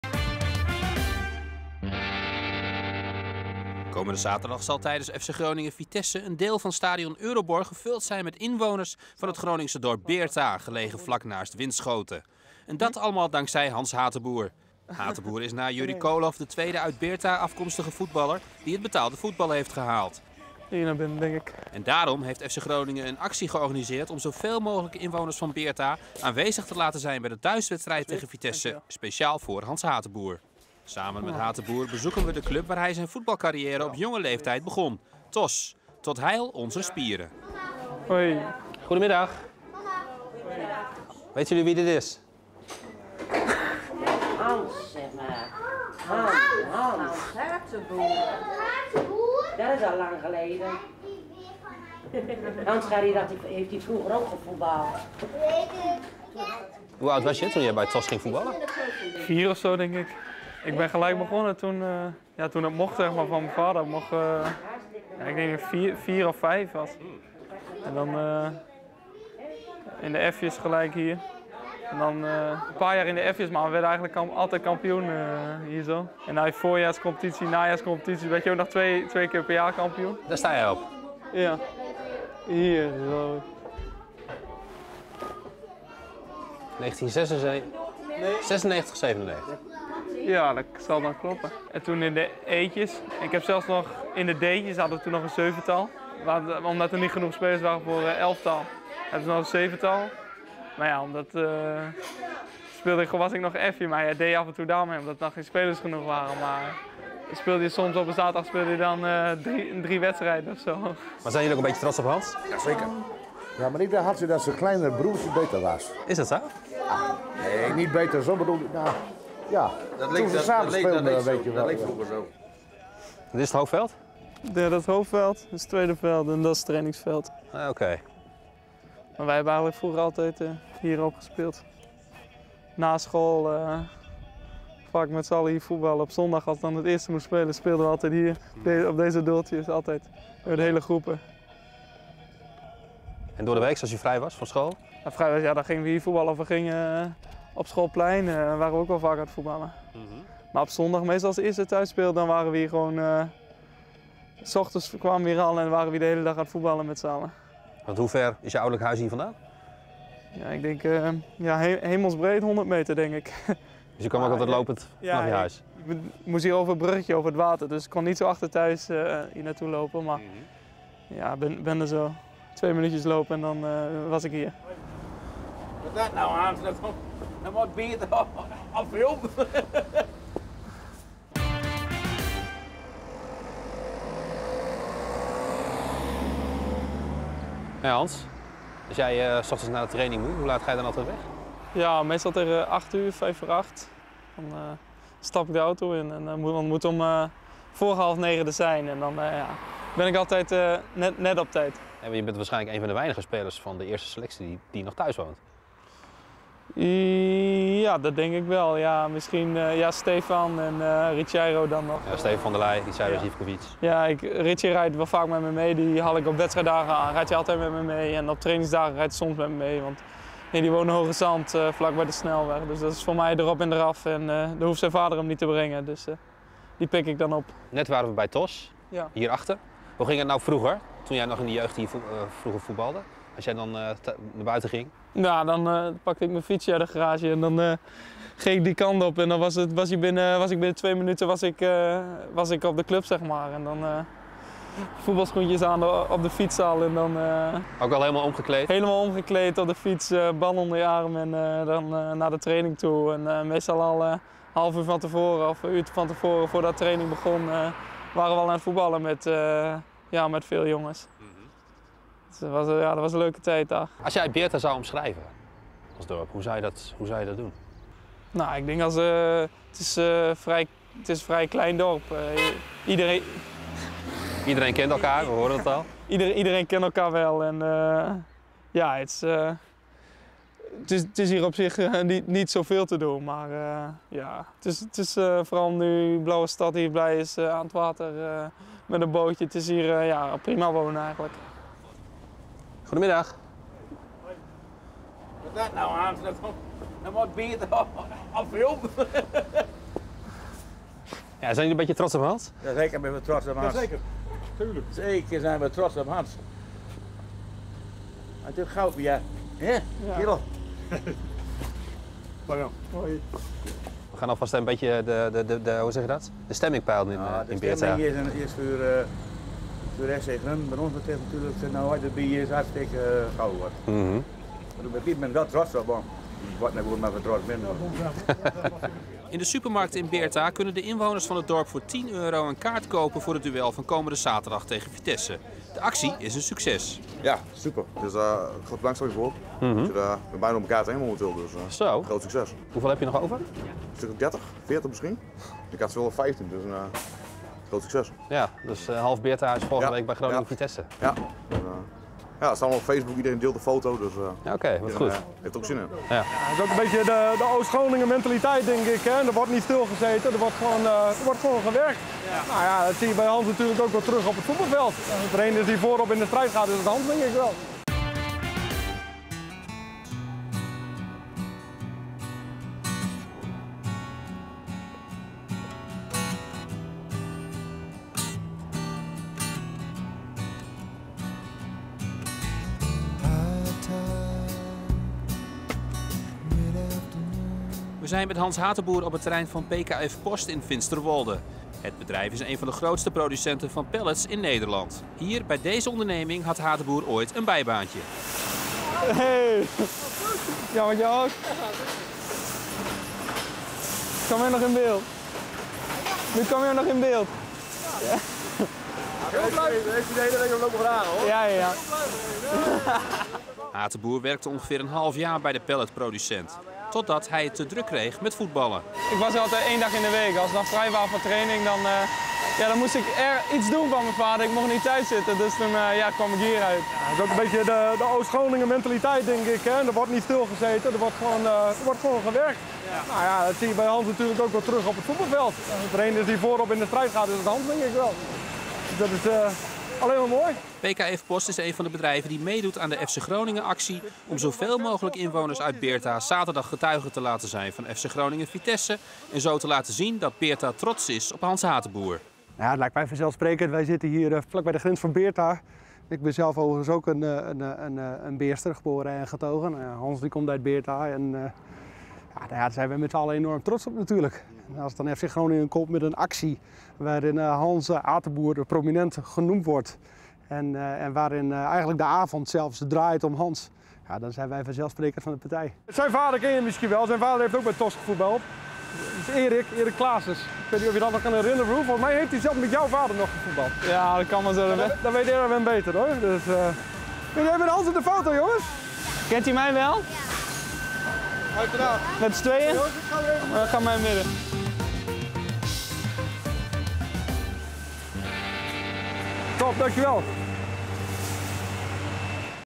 Beerta trots op Hans Hateboer
Komende zaterdag zullen veel inwoners van Beerta aanwezig zijn bij het duel van FC Groningen tegen Vitesse. Ze zullen tijdens de wedstrijd hun plaatsgenoot Hans Hateboer gaan steunen. Hateboer is na Jurrie Koolhof, tegenwoordig scout van FC Groningen, de tweede speler afkomstig uit Beerta die het betaalde voetbal heeft gehaald.

Met een speciale actie en hulp van een aantal sponsors kunnen inwoners van Beerta komende zaterdag voor 10 Euro naar het duel tegen Vitesse. Hans Hateboer gaat samen met FCG TV naar zijn woonplaats om een bezoek te brengen aan zijn oude club THOS, PKF/Post waar hij ooit een bijbaantje had en aan de C1000 in het dorp waar de kaartverkoop voor zaterdag een groot succes is.